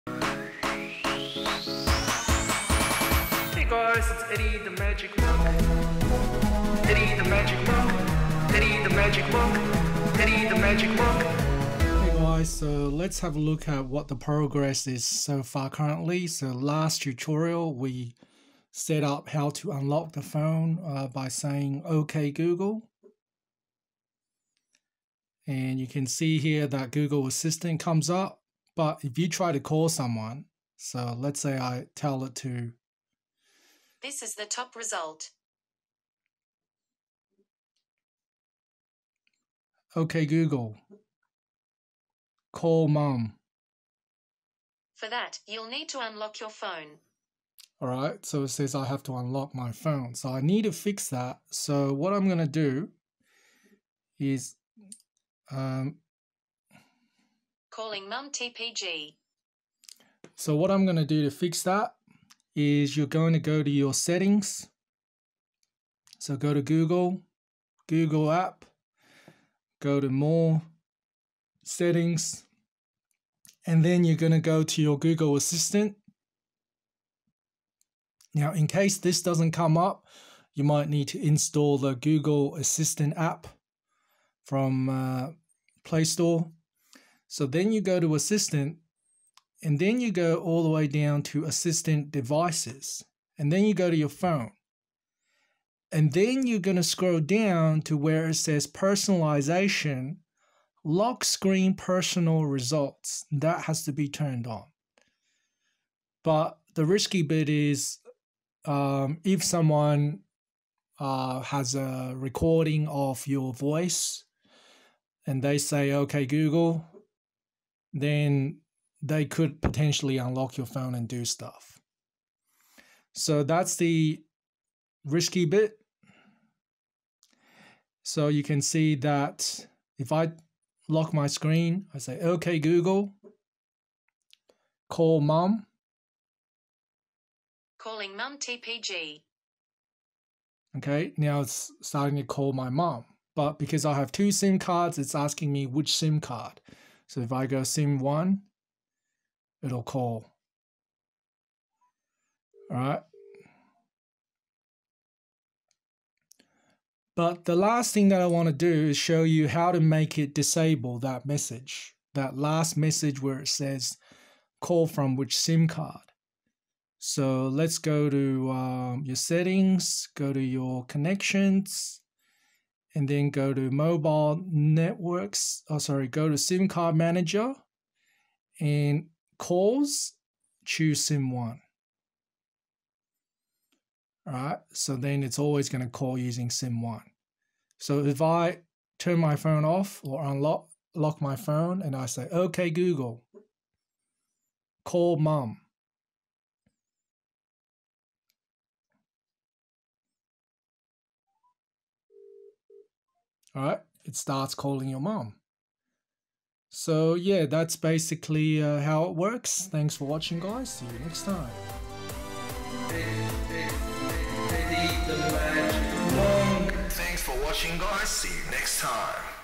Samsung Galaxy S10e part 5 - Make a call while phone is locked via Google Assitant (hands free)
So by default you can't call someone on Google Assistant without unlocking your phone first. There is a trick to that. Watch this video. Keep in mind that this could pose a security risk though if someone steals your phone and has a recording of your voice, they could access a lot more of your personal data.